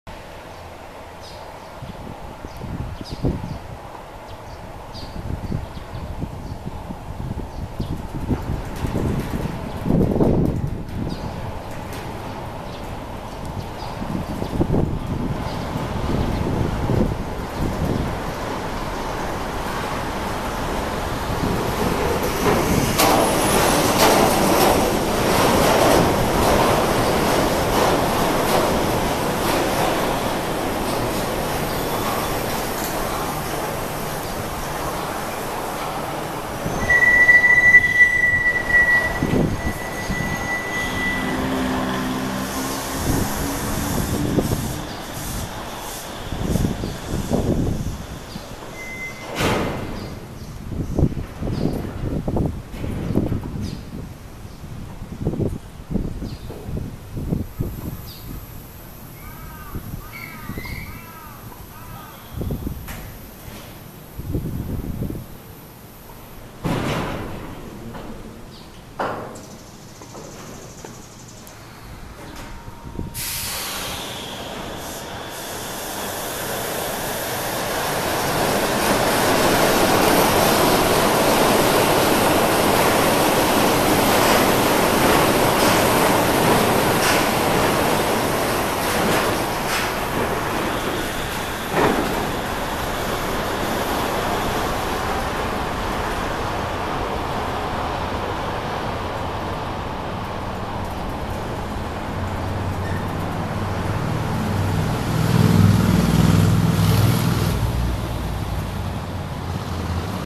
湘南モノレール目白山下駅 ポイント転換 Shonan Monorail Track Switch
湘南江の島行き電車が到着後、懸垂式モノレールの鋼鉄製桁がゆっくりと動き、進路が切り替わる。
Shonan monorail train at Mejiroyamashita station, Kanagawa Prefecture.